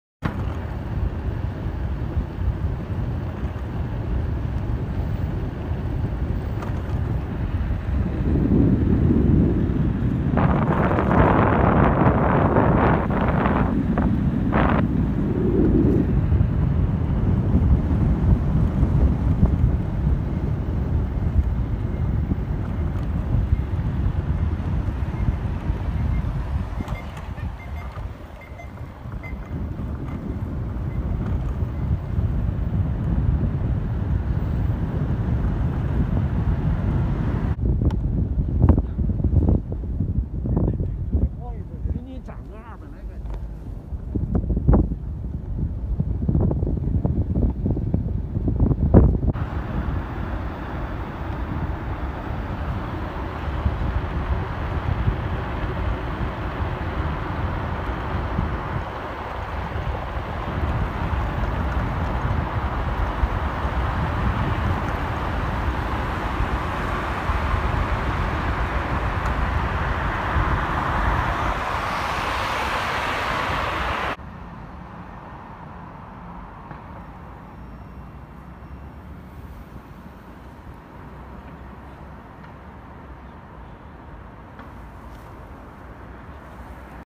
Beijing empty streets during coronavirus Outbreak in China
Beijing empty streets during coronavirus outbreak in China 2020. Beijing population: 21,540,000